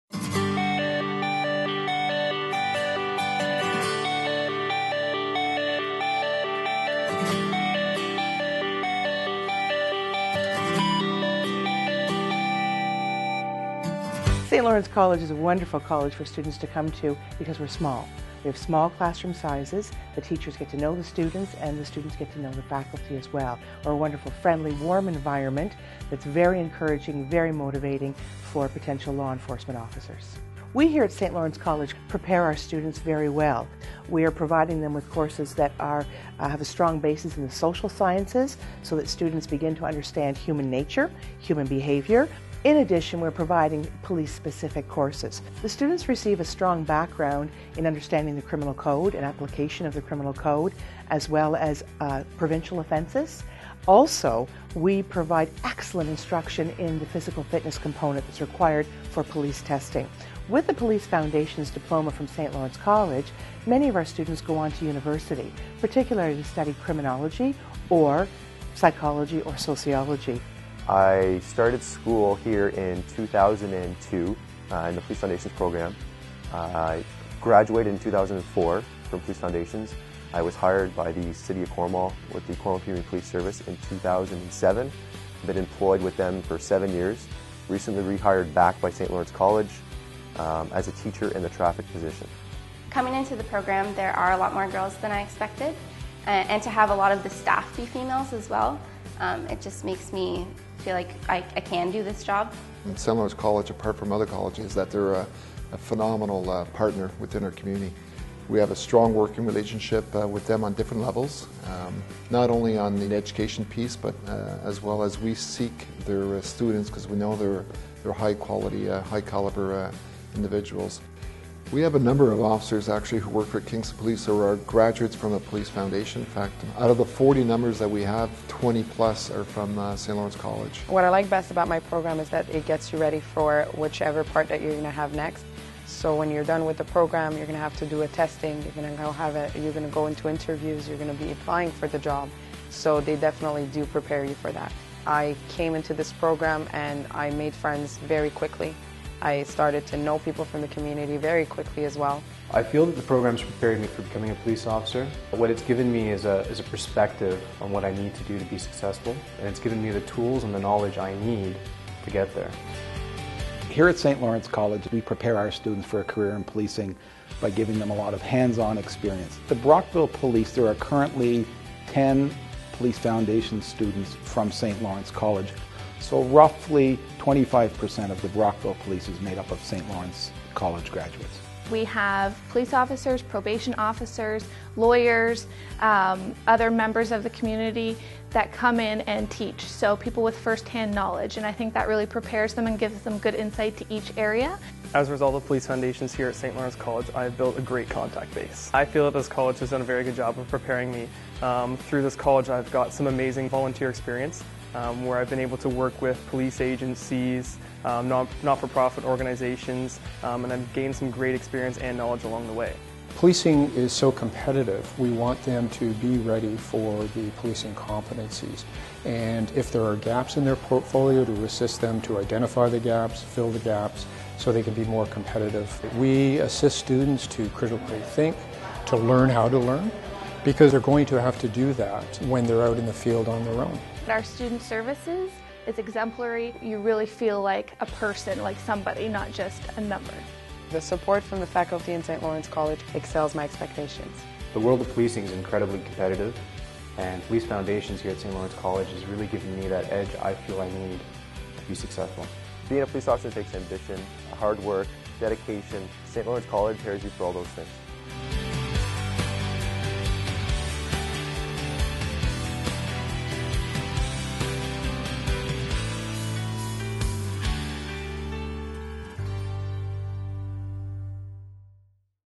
Police Foundations at SLC. Study Online or in-class in Brockville, Cornwall, and Kingston
Discover the benefits of studying Police Foundations at our three friendly St. Lawrence College campuses in Brockville, Cornwall, and Kingston. This full-time program is also offered through Online Education.